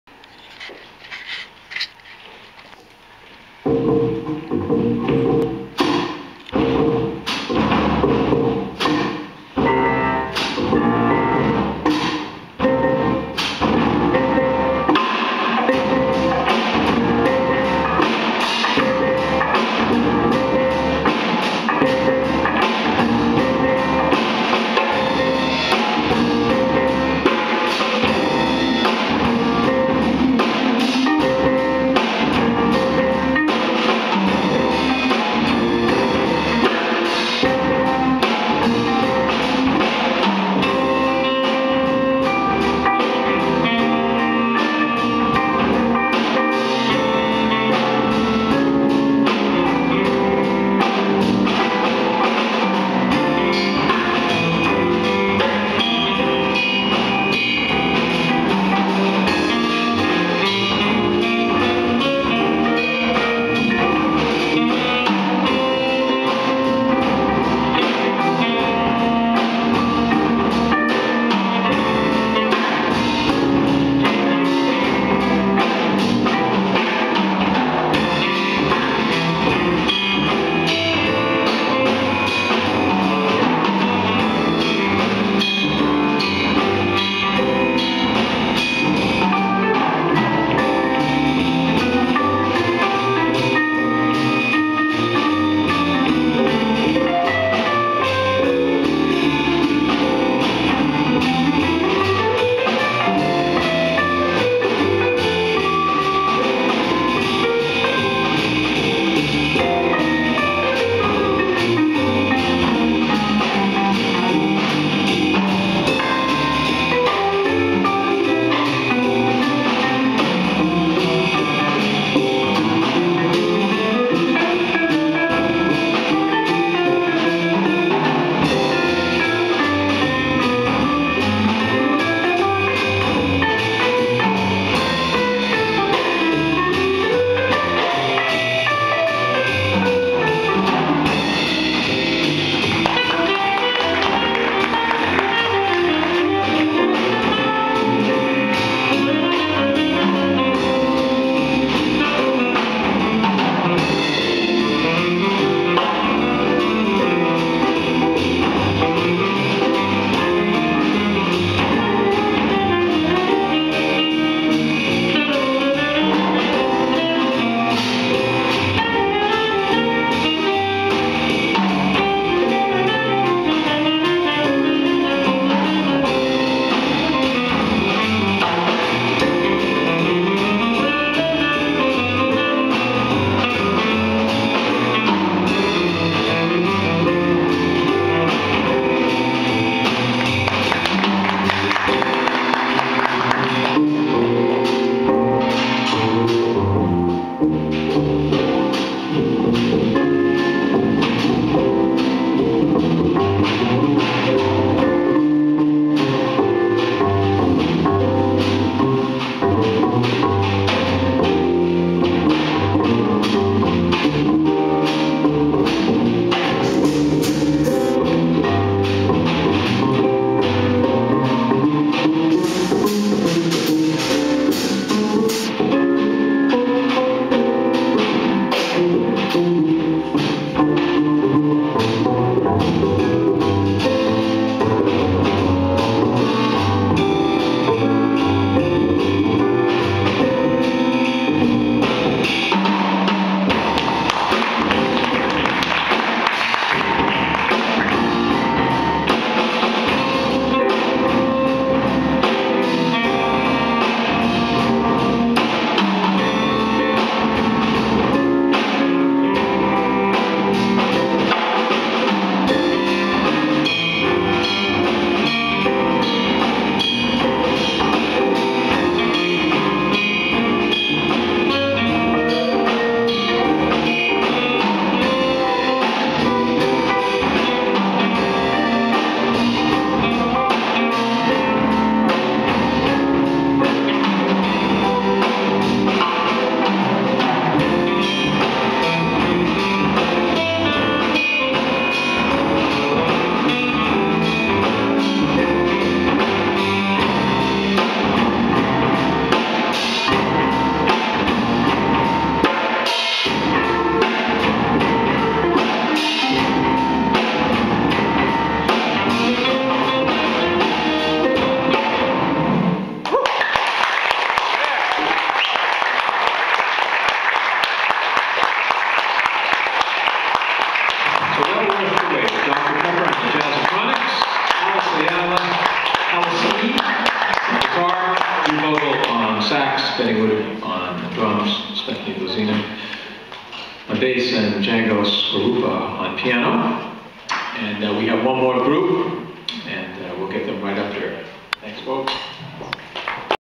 Jazz in July week 2 - Futures Concert July 22-2011 #3
Jazz in July week 2 - Futures Concert July 22-2011 #3 - Alex on guitar, Benny on drums, Django on piano, Stephanie on bass and Drew on tenor sax - UMass Amherst